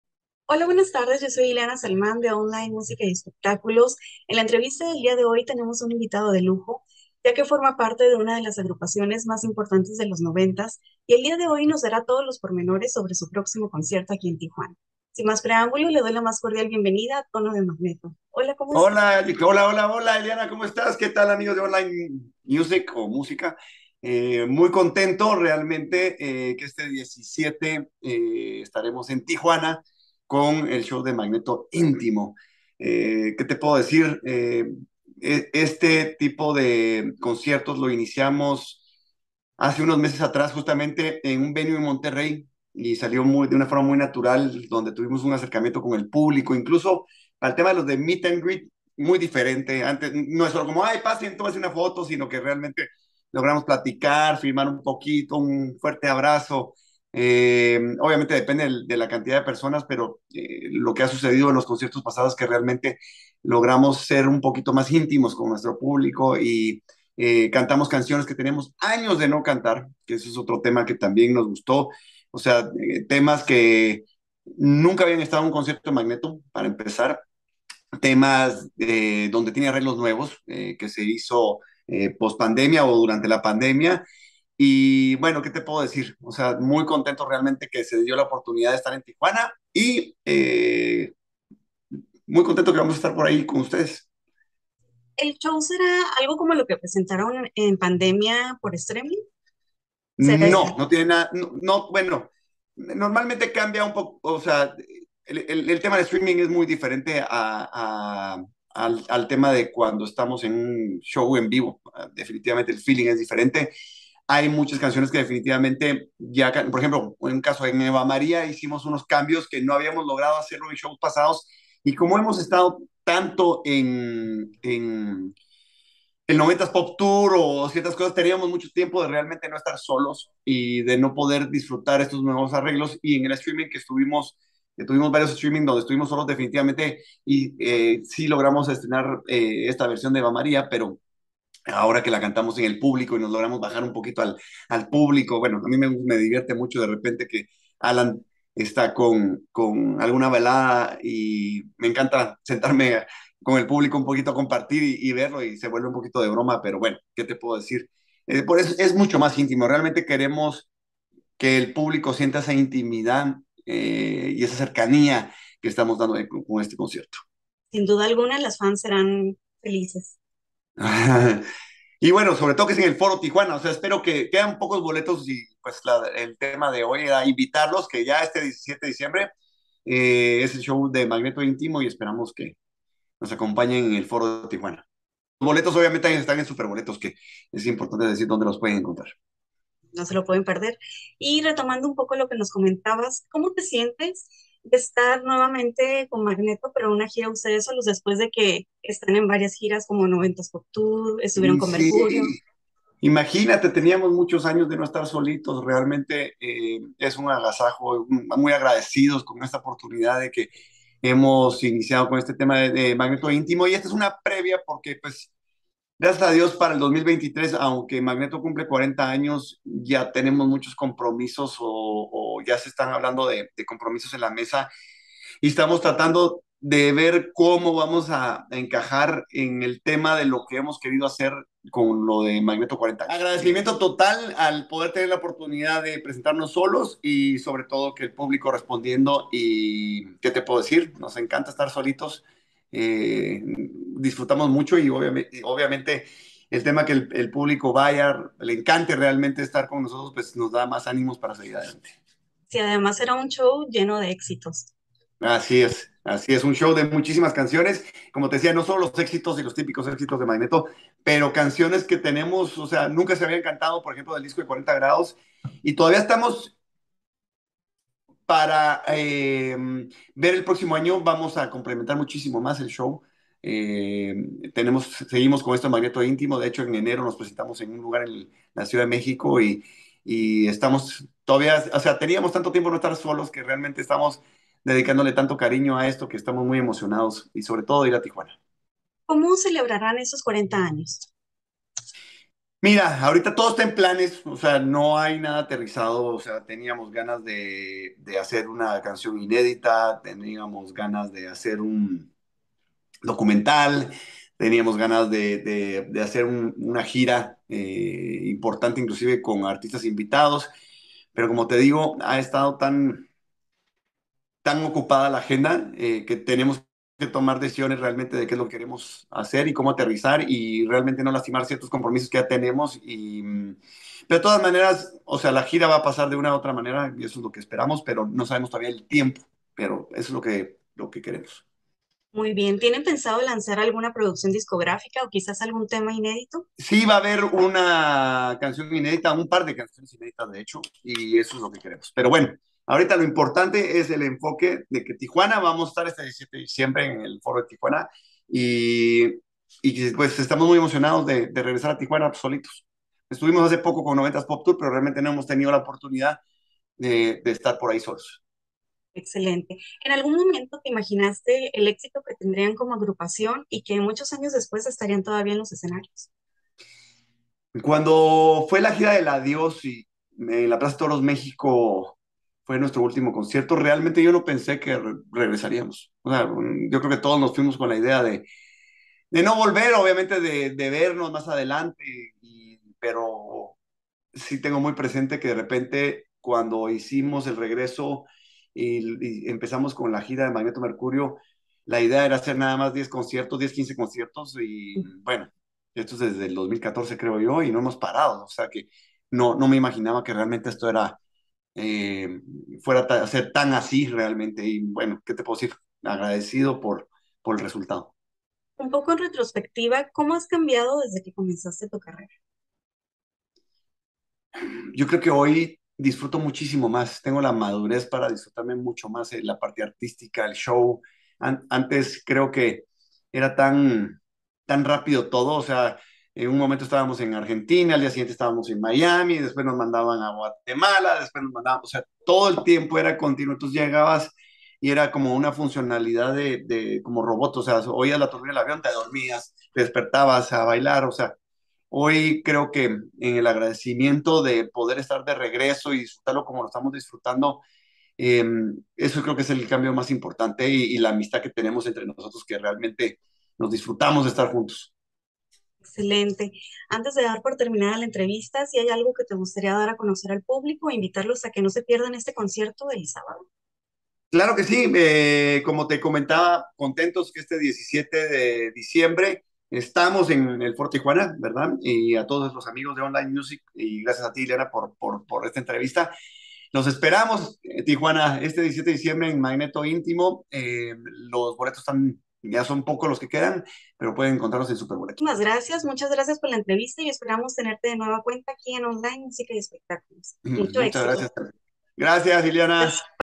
Entrevista con Tono Beltranena de Magneto sobre su concierto en Tijuana (17 de Diciembre 2022)
Entrevista con Tono Beltranena de Grupo Magneto sobre su concierto en Tijuana el próximo sábado 17 de Diciembre en El Foro Antiguo Palacio Jai Alai - Oficial

🎫 Boletos a la venta por superboletos.com